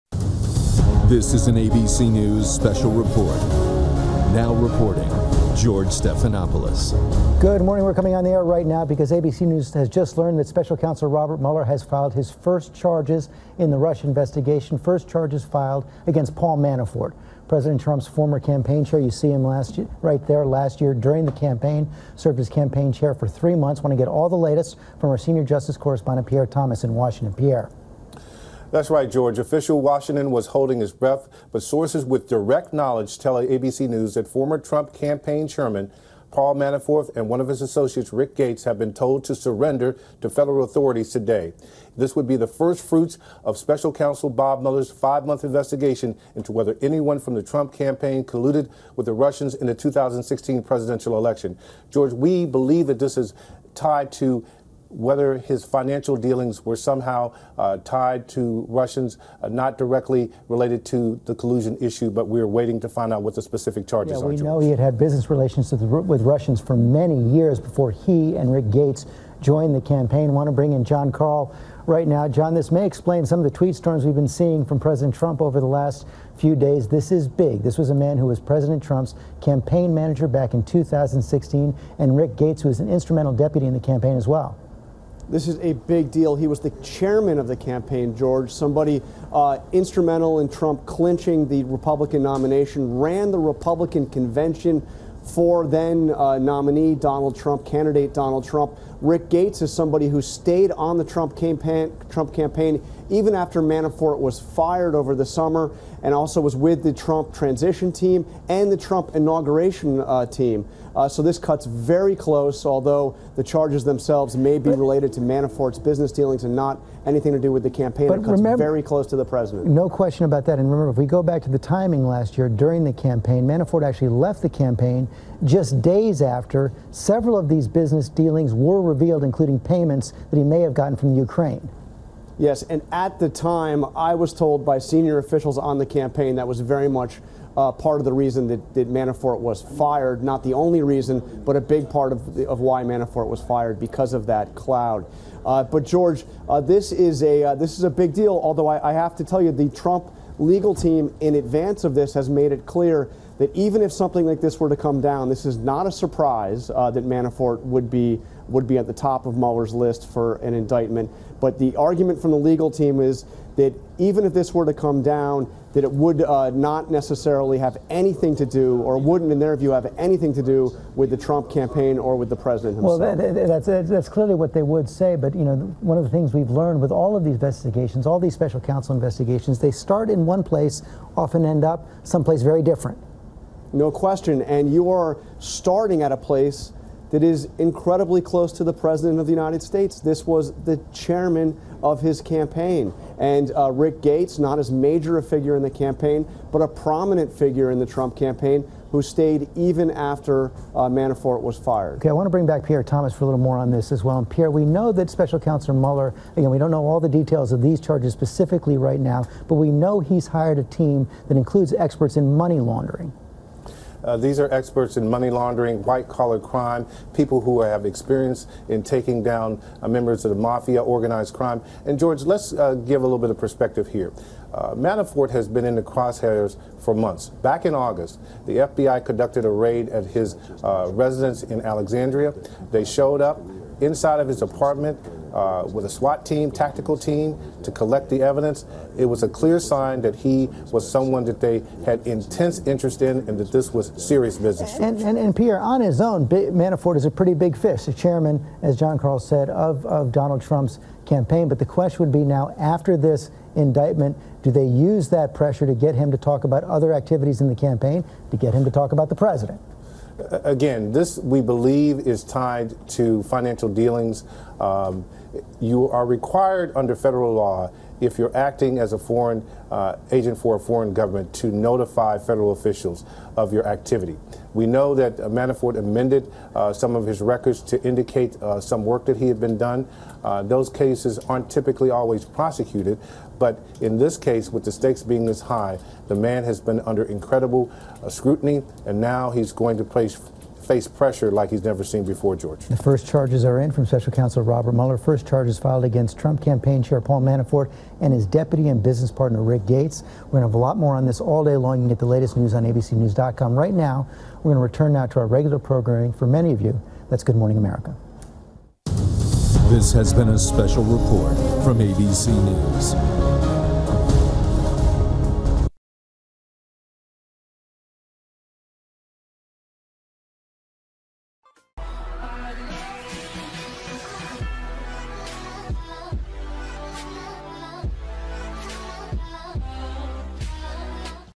Ex-Trump campaign manager to surrender to feds - NEWS TODAY
President Trump's former campaign manager Paul Manafort and Manafort's longtime business associate Rick Gates are expected to surrender to authorities today in connection to the Russia probe.